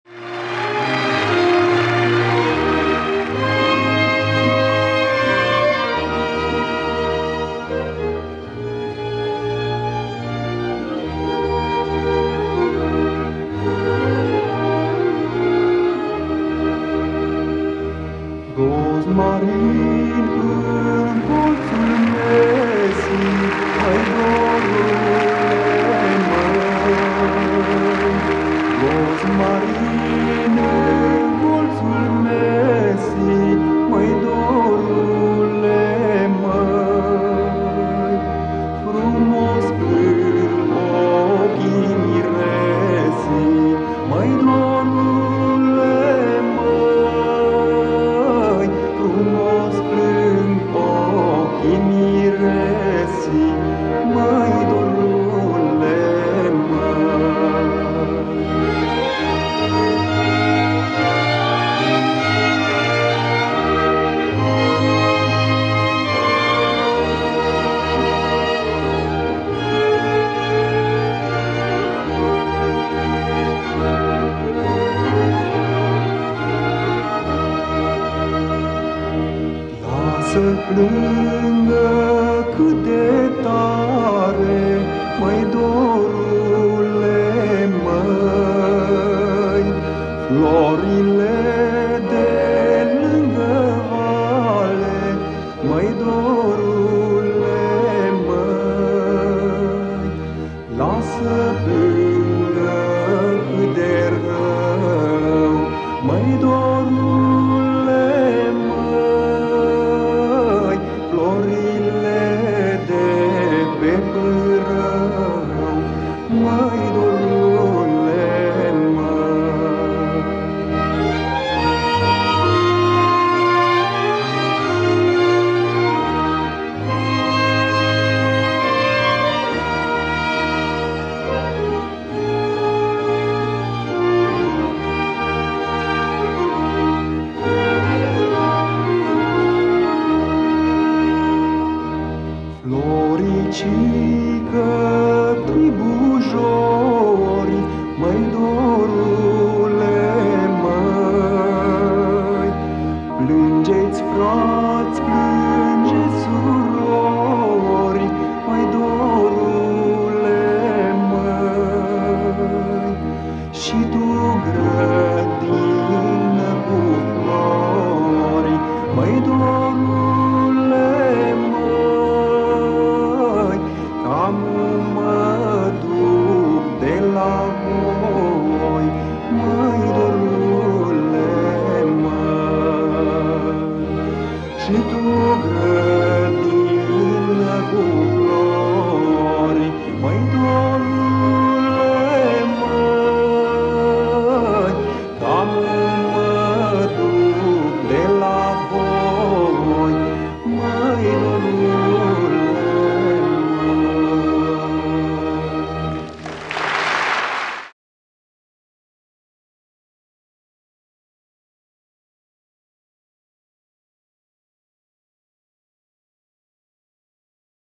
Nicolae Sabău
Emisiune de muzică populară 2004, redactor Marioara Murărescu.